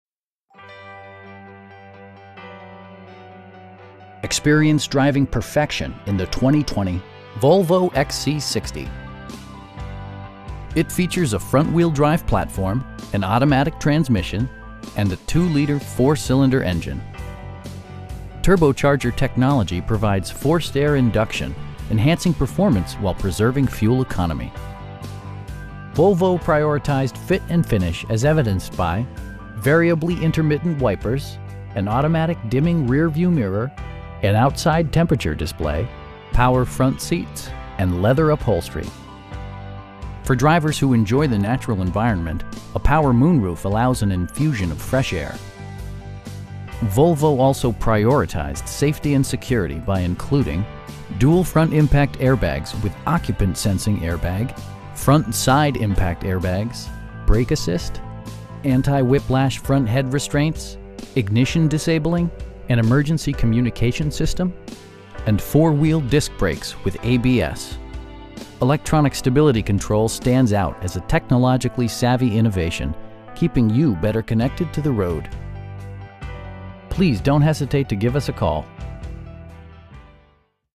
2020 Volvo XC60 T5 Inscription in Metairie, LA 70002
Bergeron Volvo Cars
3541 Veterans Boulevard

Experience driving perfection in the 2020 Volvo XC60. It features a front-wheel-drive platform, an automatic transmission, and a 2 liter 4 cylinder engine. Turbocharger technology provides forced air induction, enhancing performance while preserving fuel economy. Volvo prioritized fit and finish as evidenced by: variably intermittent wipers, an automatic dimming rear-view mirror, an outside temperature display, power front seats, tilt steering wheel, blind spot sensor, and leather upholstery. For drivers who enjoy the natural environment, a power moon roof allows an infusion of fresh air. Volvo also prioritized safety and security by including: dual front impact airbags with occupant sensing airbag, front side impact airbags, traction control, brake assist, anti-whiplash front head restraints, ignition disabling, an emergency communication system, and 4 wheel disc brakes with ABS. Electronic stability control stands out as a technologically savvy innovation, keeping you better connected to the road. We know that you have high expectations, and we enjoy the challenge of meeting and exceeding them! Please don't hesitate to give us a call.